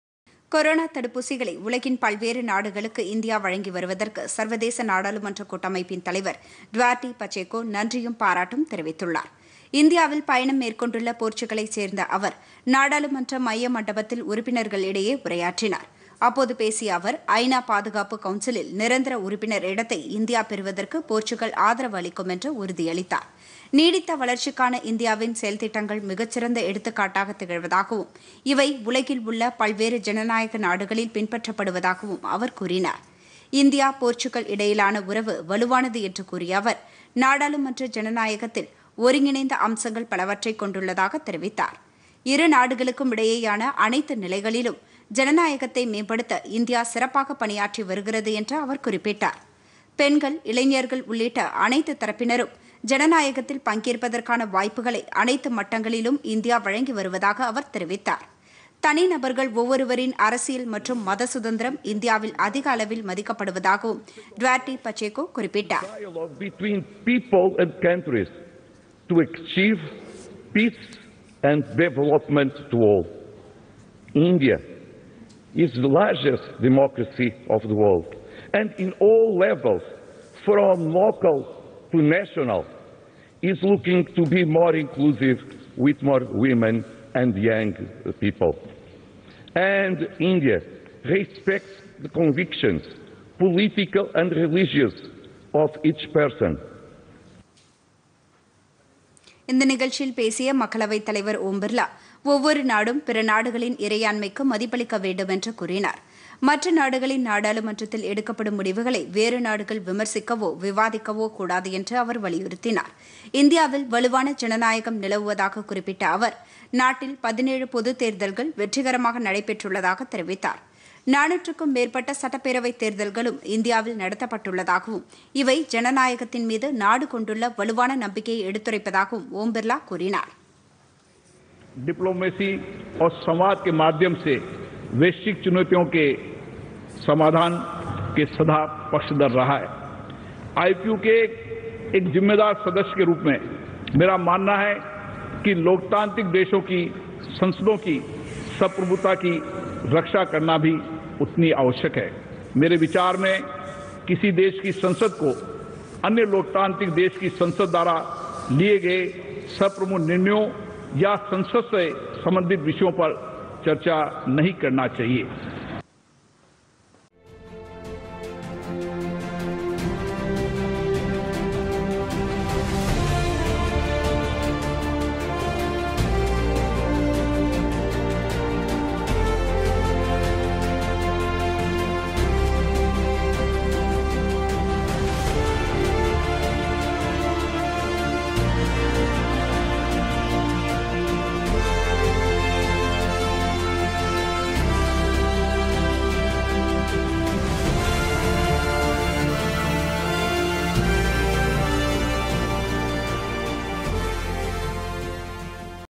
இந்தியாவுக்கு போர்ச்சுக்கல் ஆதரவுPodhigaiTamilNews
ஐ  நா பாதுகாப்பு கவுன்சிலில் இந்தியாவுக்கு  நிரந்தர உறுப்பினர் அந்தஸ்து - போர்ச்சுக்கல் ஆதரவு.